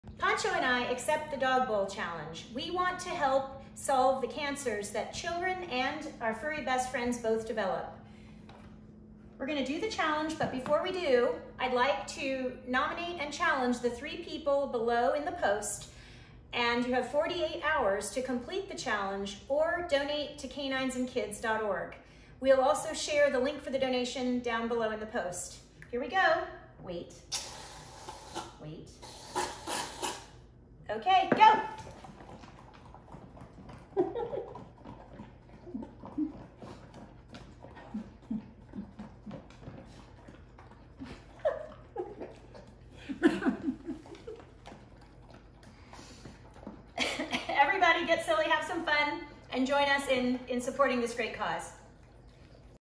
Ulrike & PANCHO Dog Bowl Challenge
DogBowlChallenge Accepted! We're trying to help raise awareness of the cancers kids and our canine best friends both develop, and incredible research that can help them both get to a cure faster!